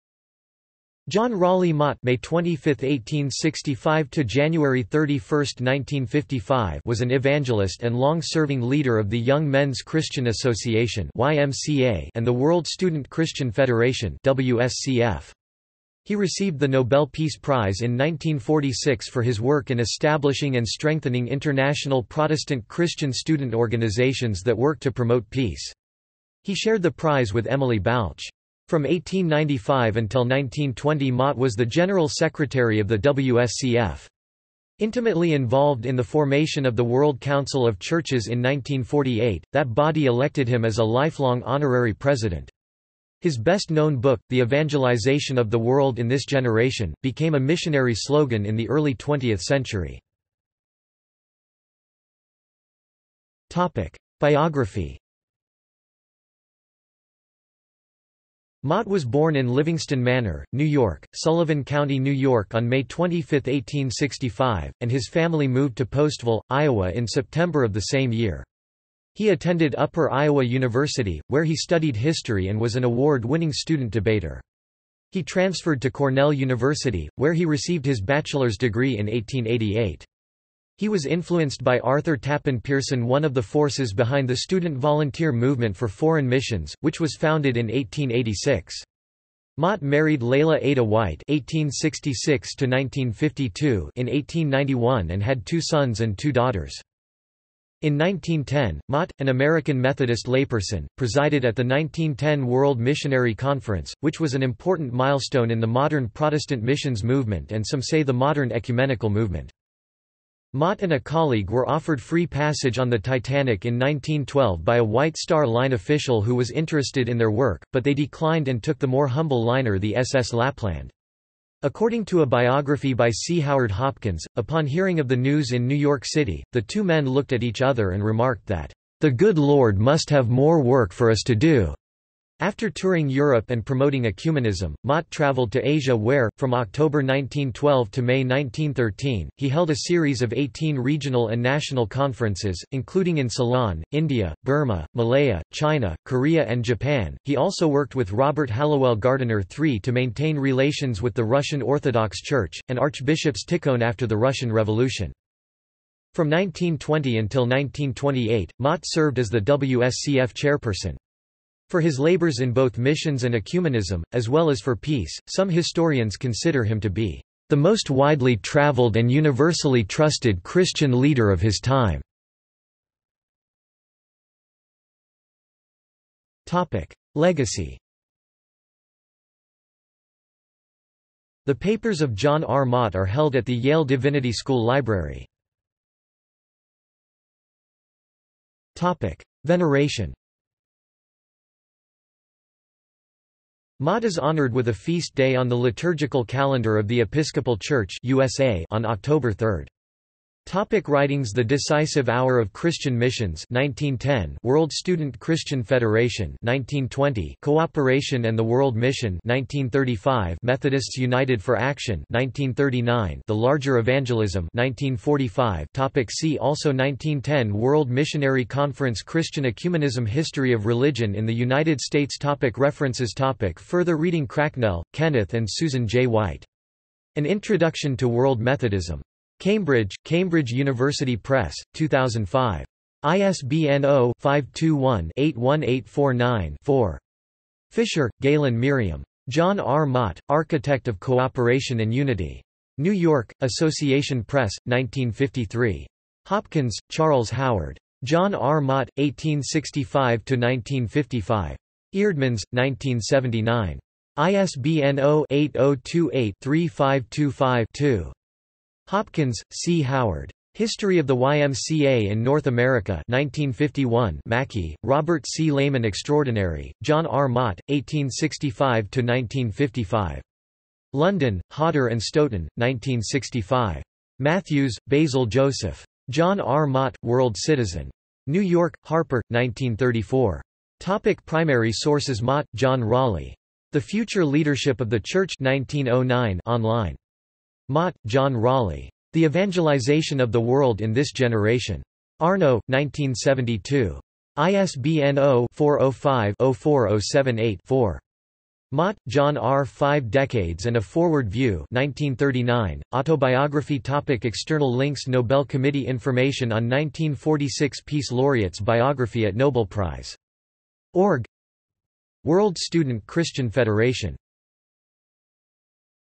John Mott | Wikipedia audio article
This is an audio version of the Wikipedia Article:
John Mott

- Socrates

SUMMARY
John Raleigh Mott (May 25, 1865 – January 31, 1955) was an evangelist and long-serving leader of the Young Men's Christian Association (YMCA) and the World Student Christian Federation (WSCF).  He received the Nobel Peace Prize in 1946 for his work in establishing and strengthening international Protestant Christian student organizations that worked to promote peace. He shared the prize with Emily Balch. From 1895 until 1920 Mott was the General Secretary of the WSCF. Intimately involved in the formation of the World Council of Churches in 1948, that body elected him as a lifelong honorary President.  His best-known book, The Evangelization of the World in this Generation, became a missionary slogan in the early 20th century.